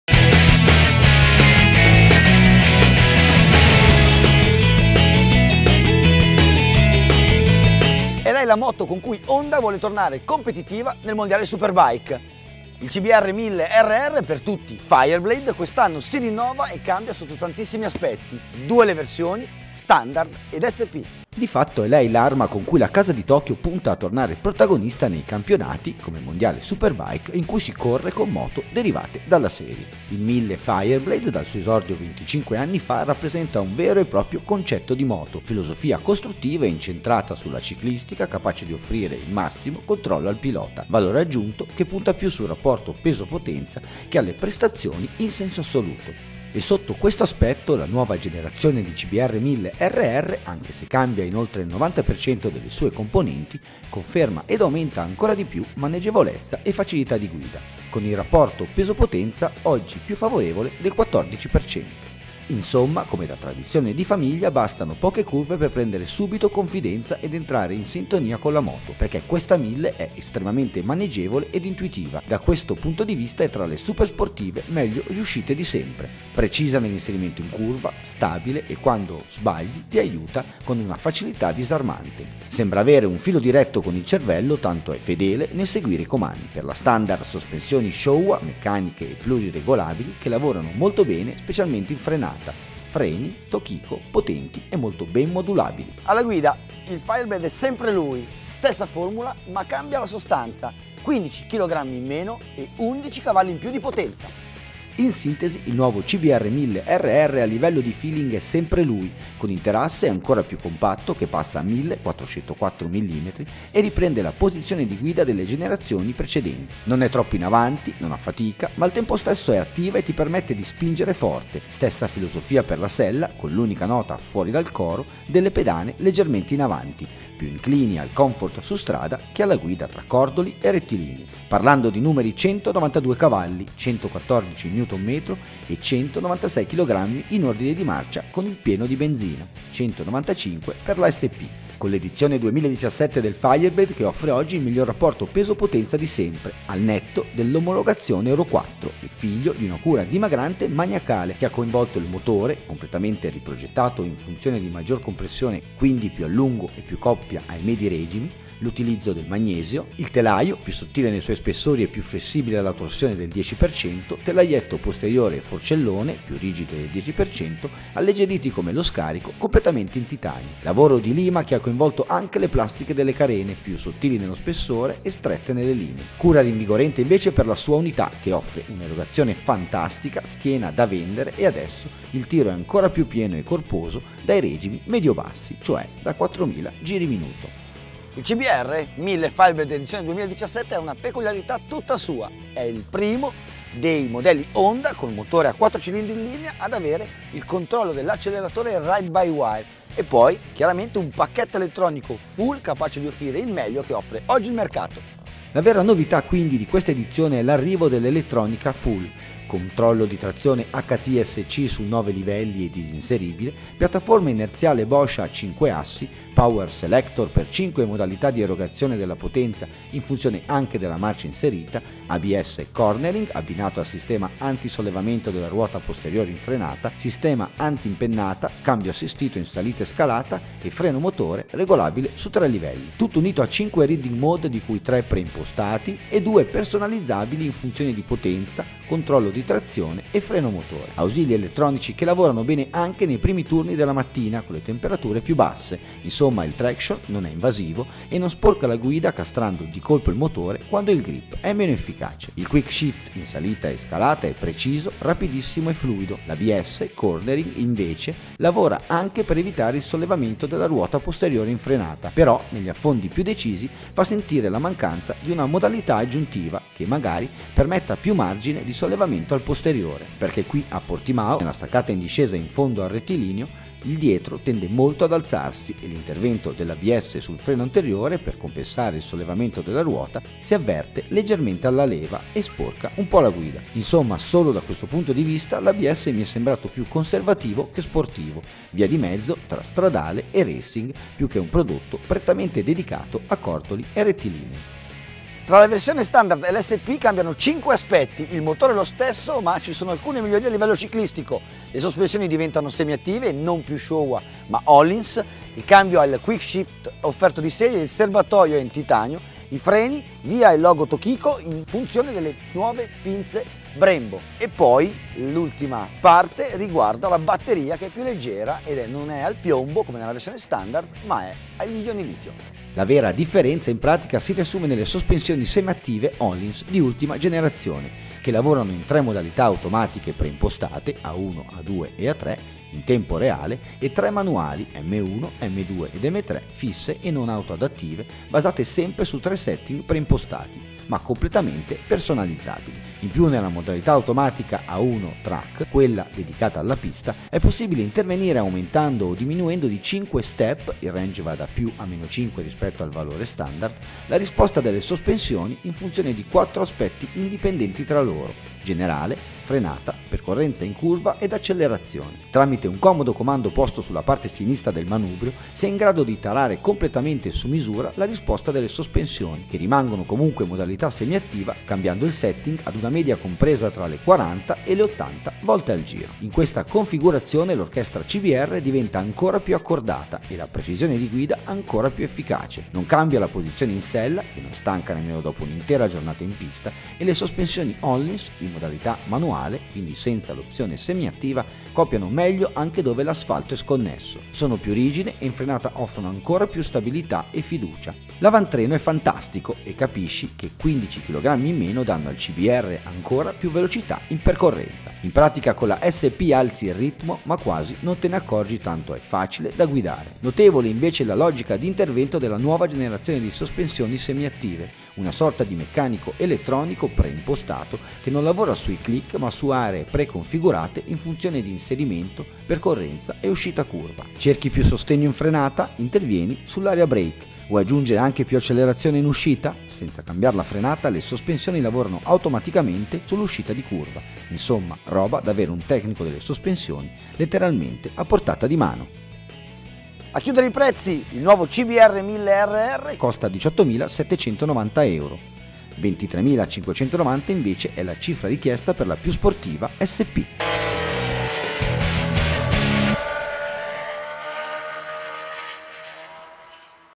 Honda CBR1000RR Fireblade ed SP my 2017
La prova del nuovo CBR1000RR Fireblade MY 2017 in versione standard e più sportiva SP!